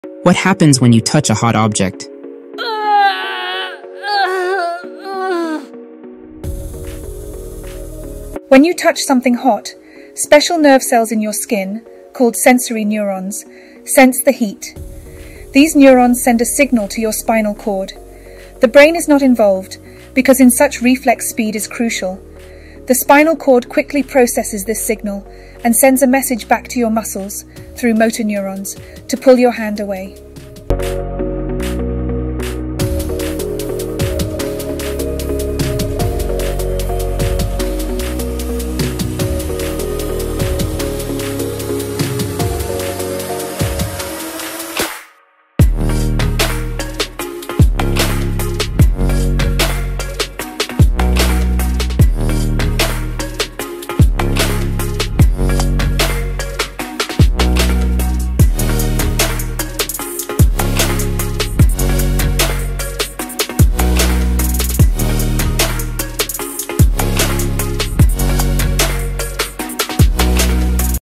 Does Your Body REALLY React INSTANTLY When You Touch Something HOT
Explore the fascinating world of 'reflex actions' and how your body responds instantly! This video explains how 'sensory receptors' trigger a rapid response through the 'reflex arc', bypassing conscious thought. Learn about the crucial role of the 'spinal cord' in these lightning-fast reactions, and how 'neurons' communicate.